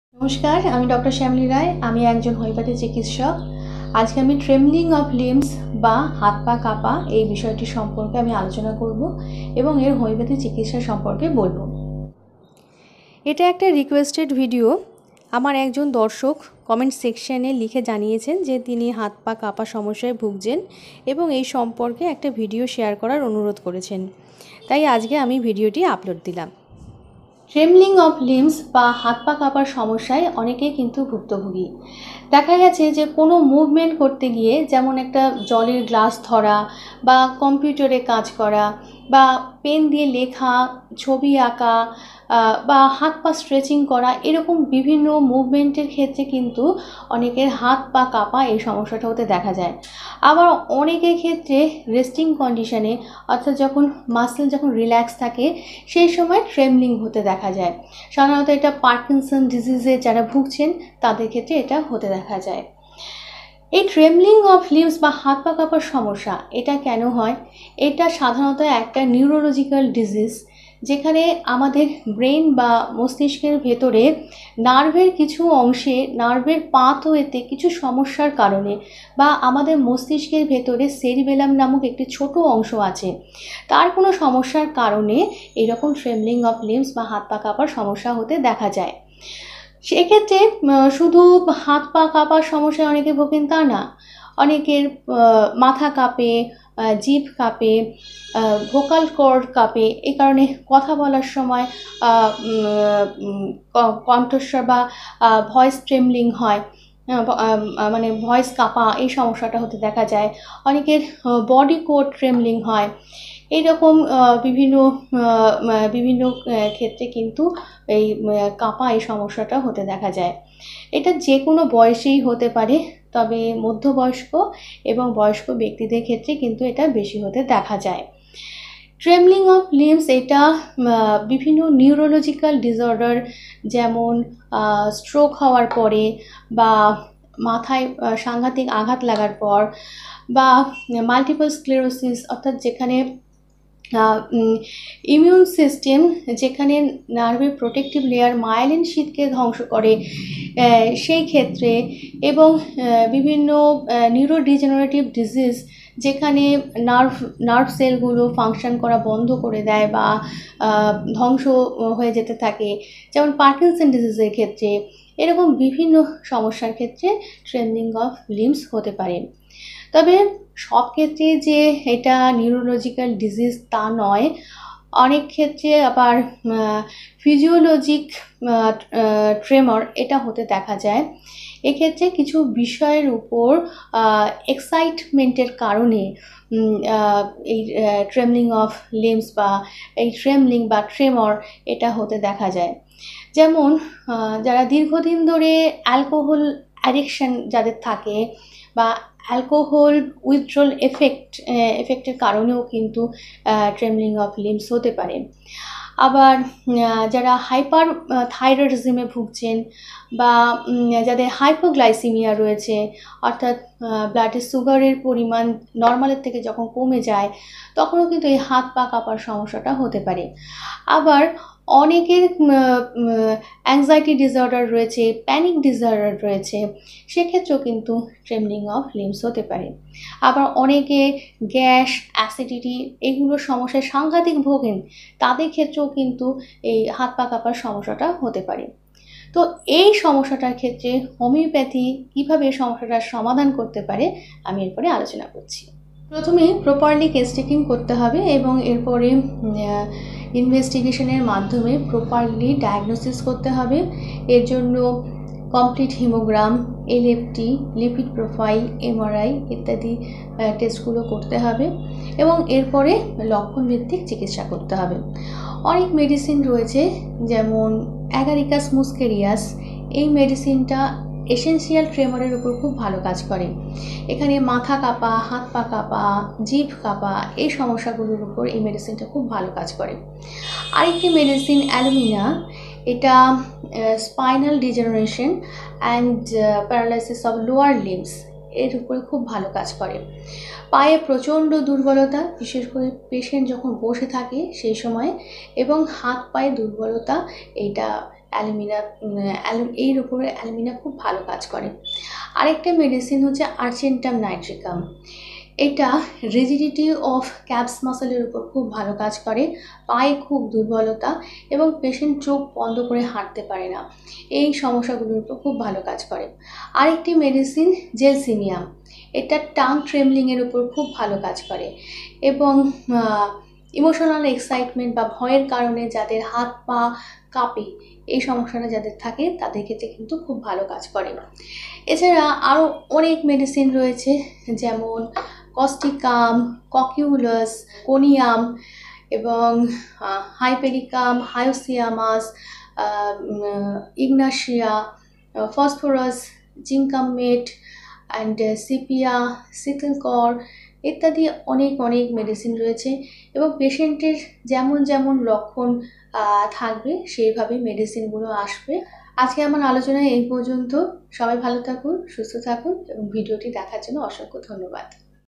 হাত পা কাঁপছে?Trembling Of Limbs?কারণ কি? হোমিওপ্যাথি চিকিৎসা সম্পর্কে জানতে অবশ্যই দেখুন ভিডিওটি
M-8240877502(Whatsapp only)